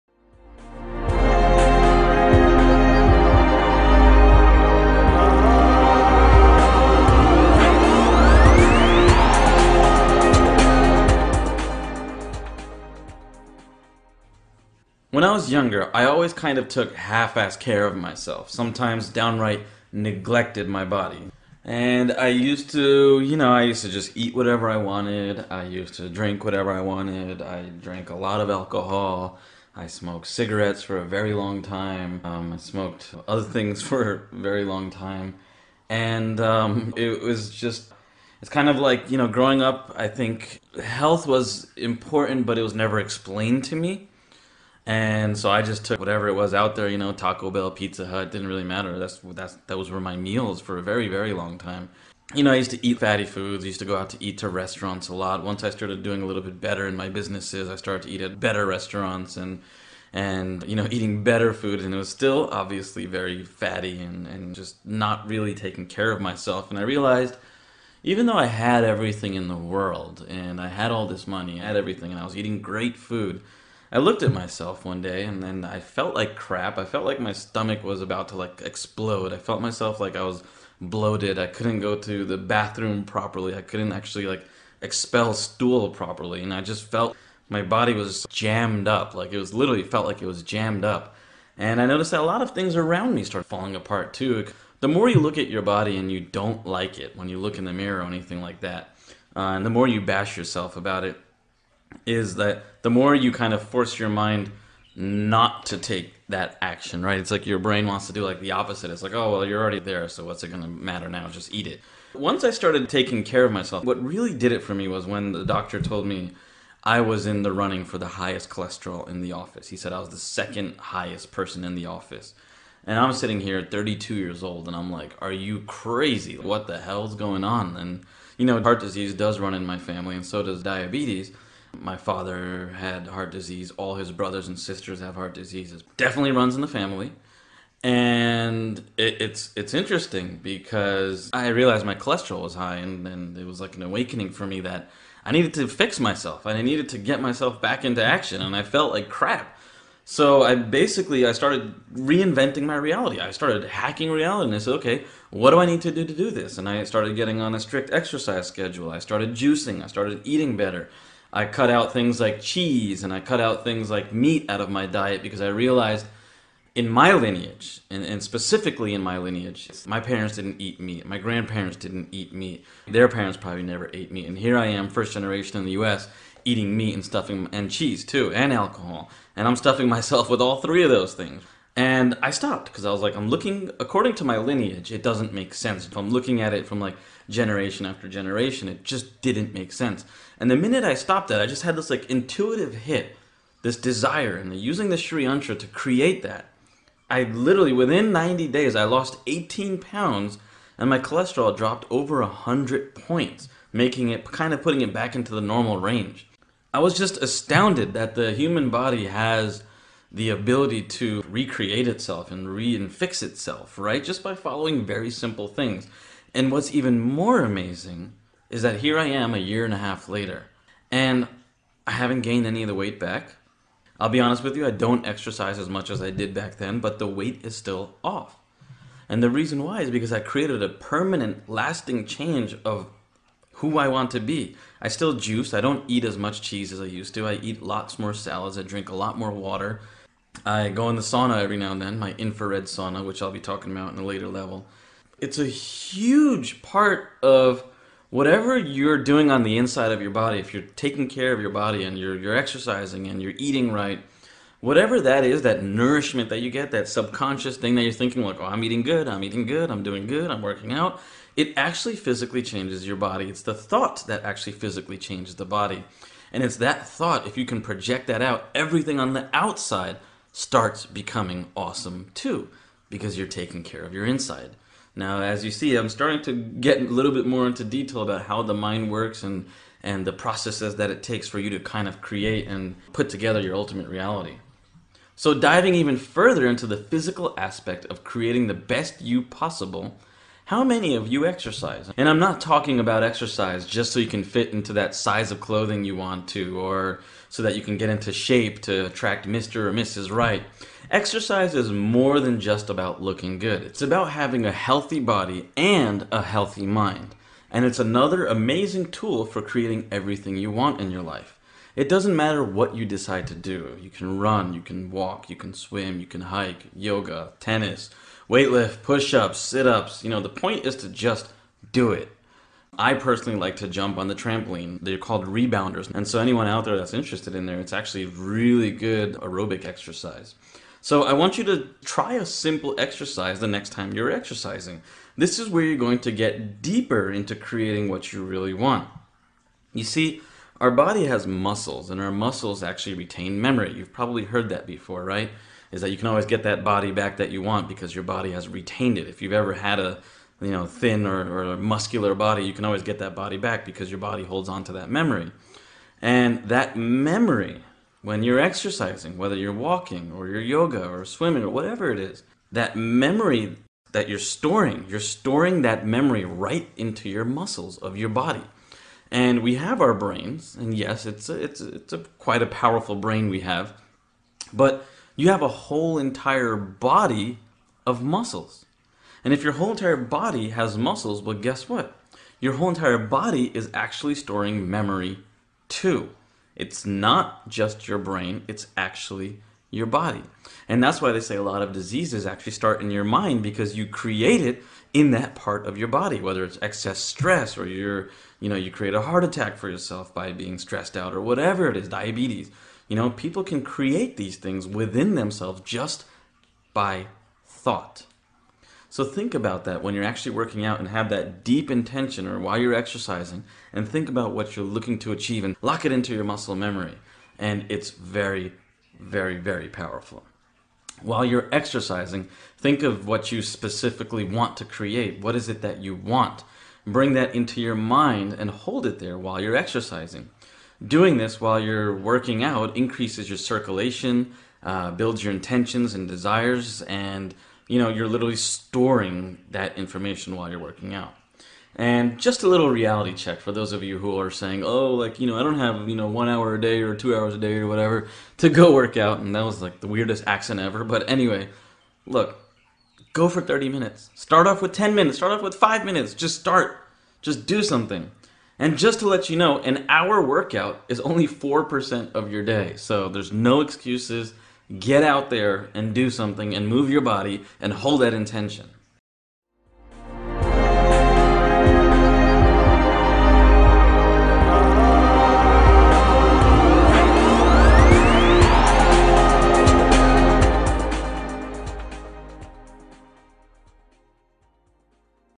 Spiritual Growth Physical Exercise
Exercise is more than just about losing weight to fit into the size of clothing you want or looking good for another person. It’s about having a healthy body and a healthy mind...and it’s another amazing tool for creating everything you want in life. Physical exercise is very important for our health. It builds strength, motivate us and make us focused on activities. It also helps us grow spiritually!

Please check out the link to learn more about how to change your life for the better and unleash your maximum potential!